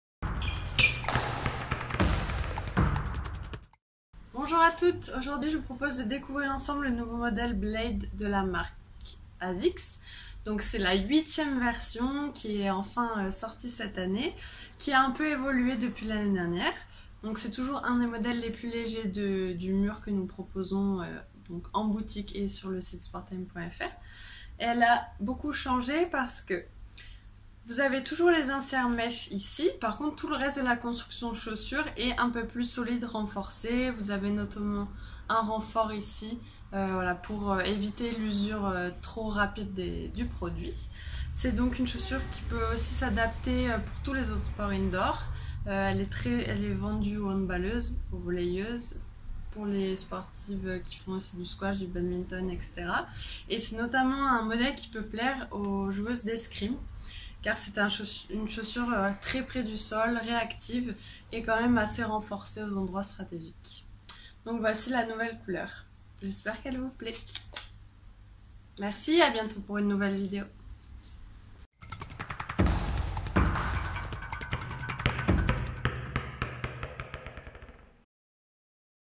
Chaussures squash et badminton asics blade femme
Les chaussures squash et badminton asics blade sont de retour avec une 8ème version qui change beaucoup. Profitez toujours d'une légèreté absolue, mais avec des matériaux plus solides et renforcés, notamment au creux du pied avec le système P-Guard. Idéales pour tous vos sports indoor !